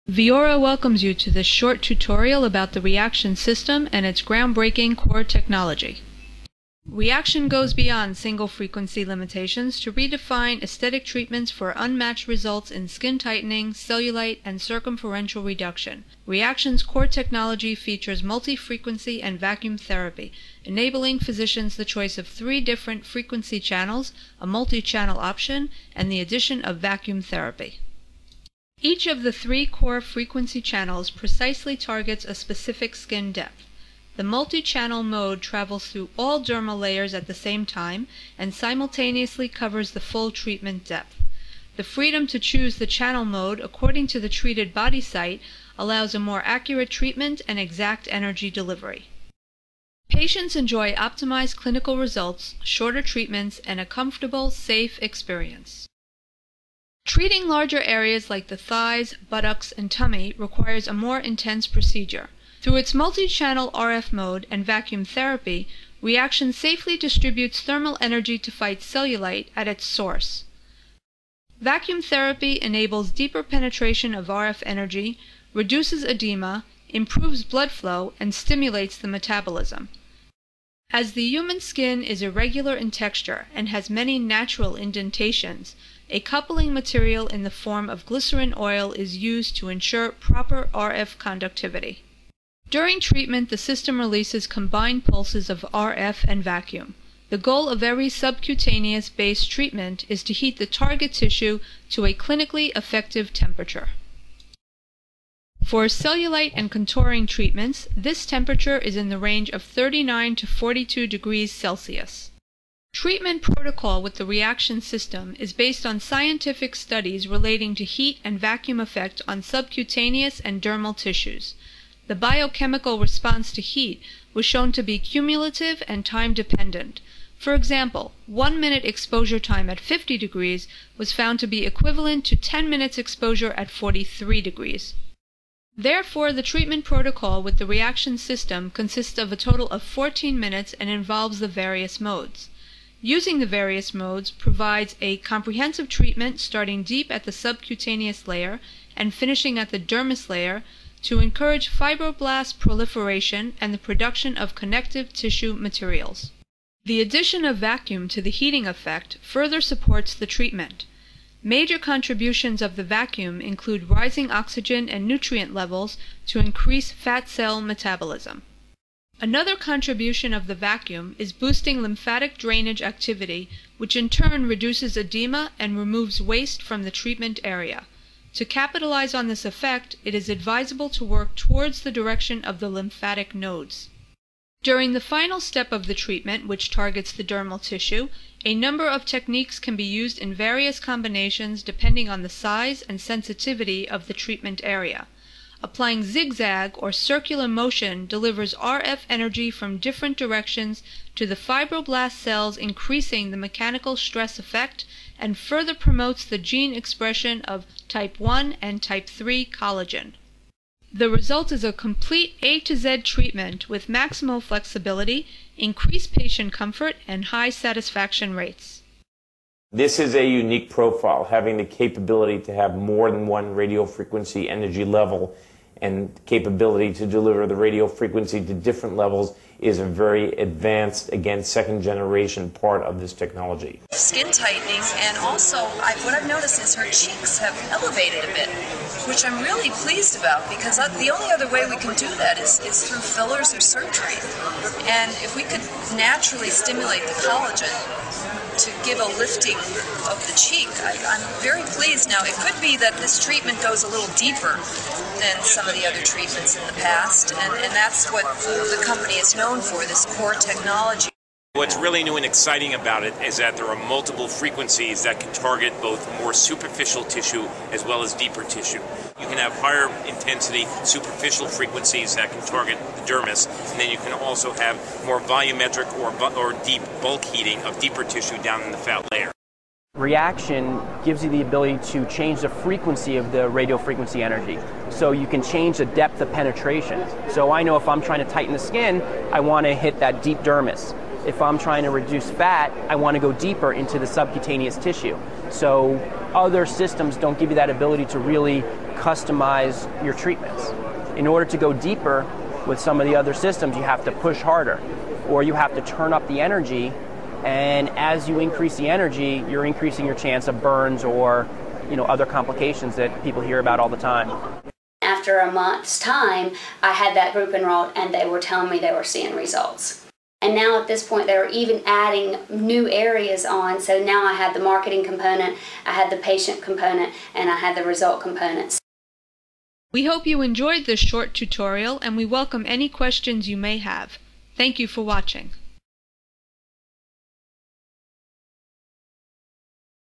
מכשיר ריאקשן - מפי המומחים
מצגת על יתרונות מכשיר הריאקשן מפי מומחים בתחום ורופאים. המצגת כוללת בתוכה הסבר כללי על המכשיר, הסבר על הטכנולוגיה ועדויות.